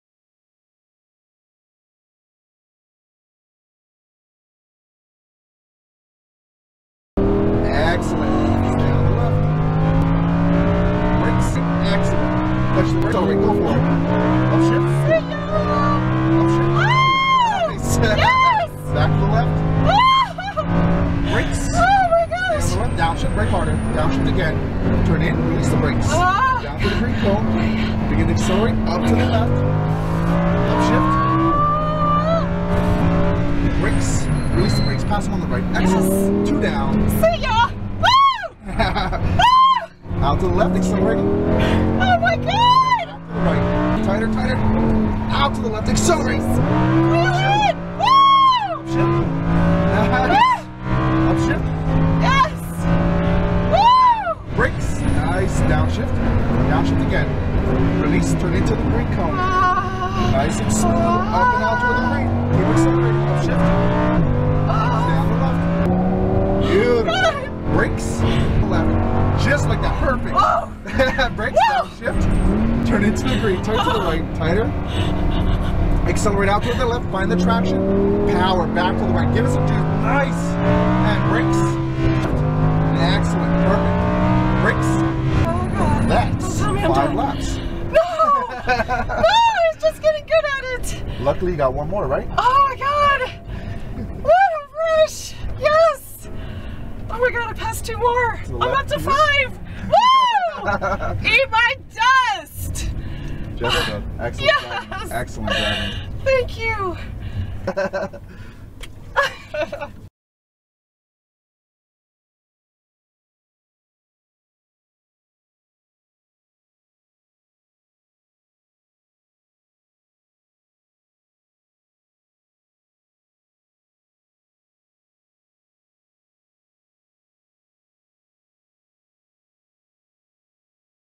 Favorite Moments in the Green Huracan with Abraham at Exotics Racing
Passed a Lambo and a Ferrari at Exotics!   Best. Day. Ever. Lap time 57:18 with top speed around 120 mph.  5 laps is not enough!